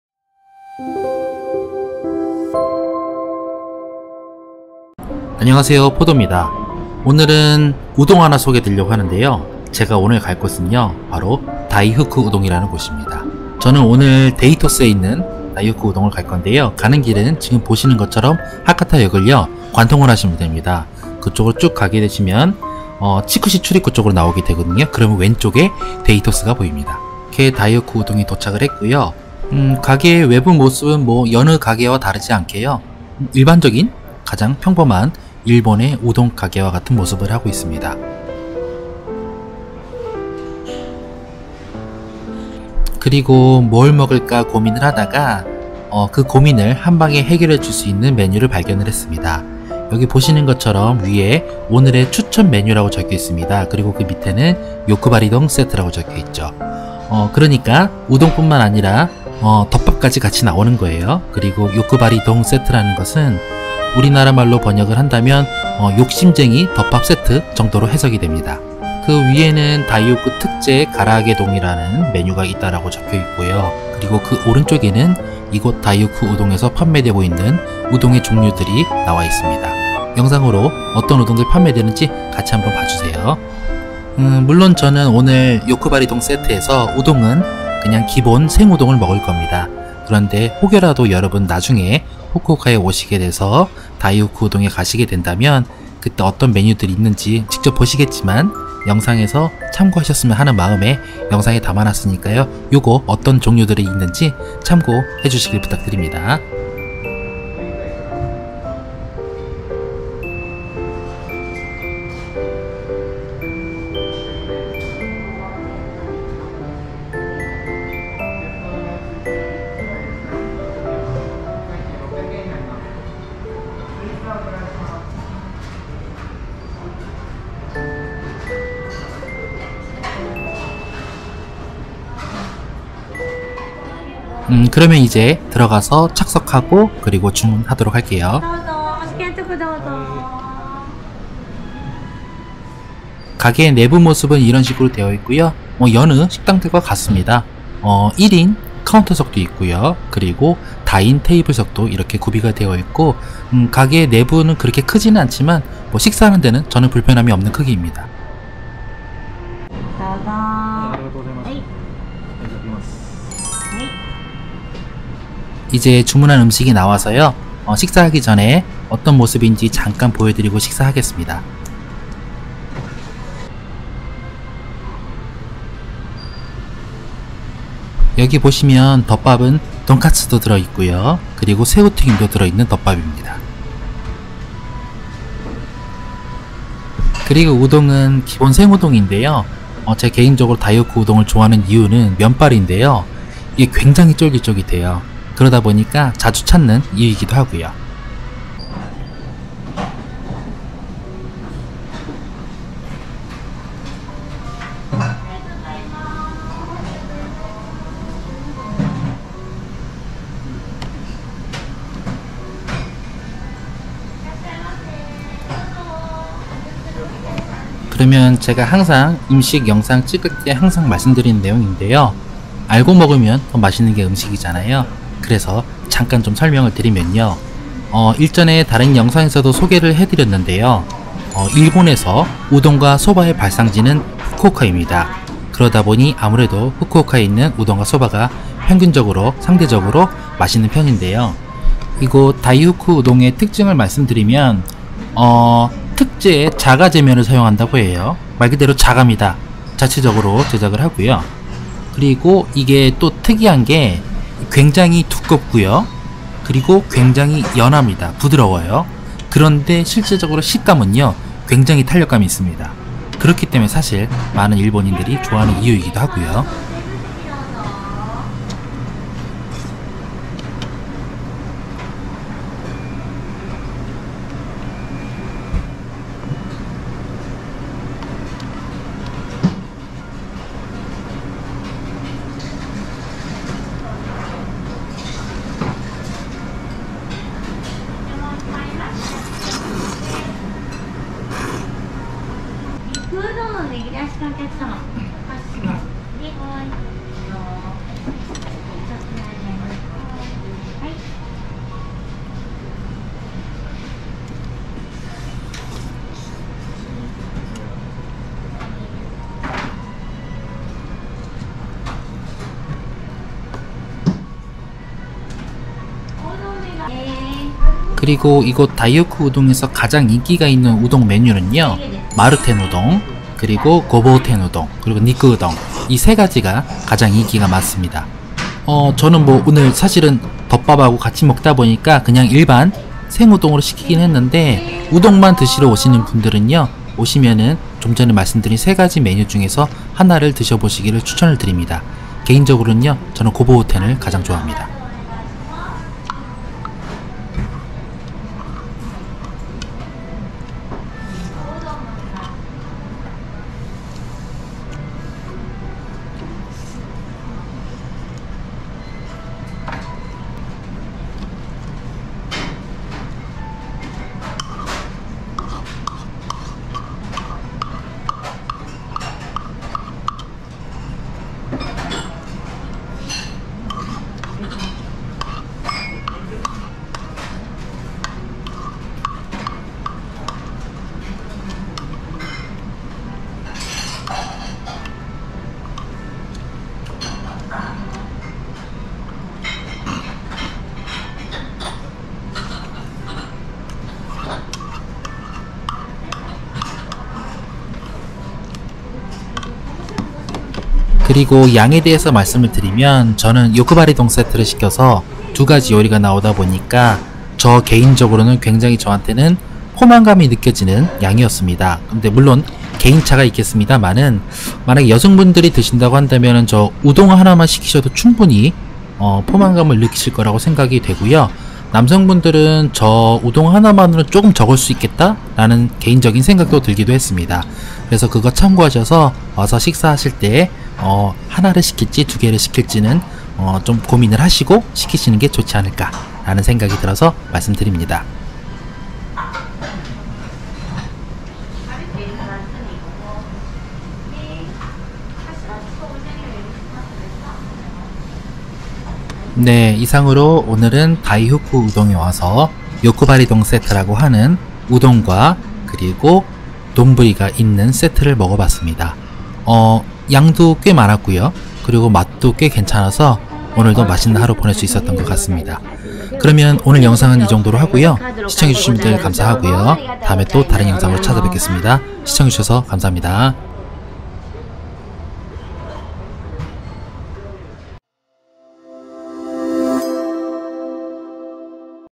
🍜일본 후쿠오카 우동 맛집 다이후쿠 우동을 소개해 드려요 / 일본 맛집 소개 / 일본 현지 거주 한국인 일상 / 화질 1440p 권장
✈ 오늘은 오랜만에 맛있는 일본여행 컨텐츠로 찾아뵈요.

📺오늘은 일본 내 우동의 발상지인 후쿠오카에서도 우동 맛집으로 유명한
다이후쿠 우동에서 우동을 먹고 왔습니다.

📺이곳 다이후쿠 우동의 특징은 두껍고 탄력성이 높아서 식감이 쫄깃쫄깃
하다는 것인데요. 이러한 특징 인해서 많은 현지 일본인들로부터 사랑을 받고
있습니다.

하여, 이번에 여러분께 다이후쿠 우동을 촬영하여 보여드리려고 합니다.

그러면 상세한 내용은 영상 통해서 말씀 드릴께요^^

♥다이후쿠 우동 데이토스점 정보 :
① 위치 : 〒812-0012 Fukuoka, Hakata Ward, Hakataekichuogai, 1−1
② 영업시간 : 07:30~22:30 (평일・주말 동일)